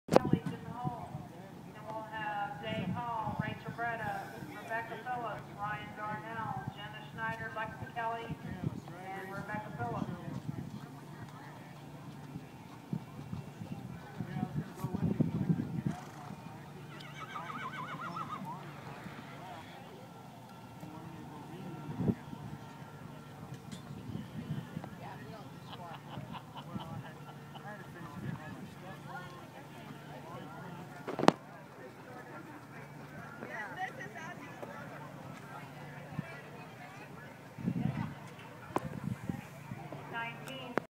NBHA: Aztec trainers lope 19.684!! 8-10-13
Our fastest time yet, and we aren't adding speed yet!!  So proud of my boy!  He has come so far already and I cannot wait to see where we go! :D